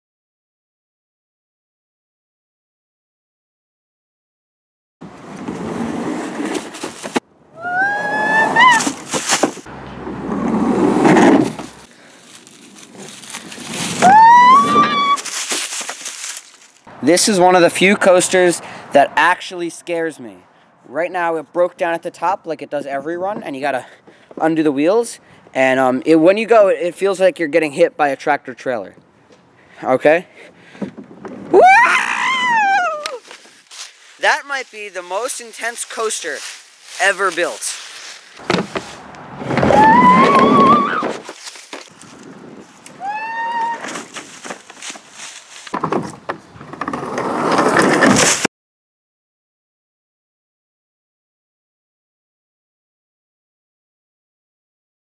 Leaf Coaster Roller Coaster POV Ridercam Off Ride Reverse POV AWESOME HD Footage - Leaf Park
Originally built in 2005, Leaf Coaster, West Orange's older roller coaster located at Leaf Park, is showcased in this video.

Footage by Truman Segal and Louis Kravits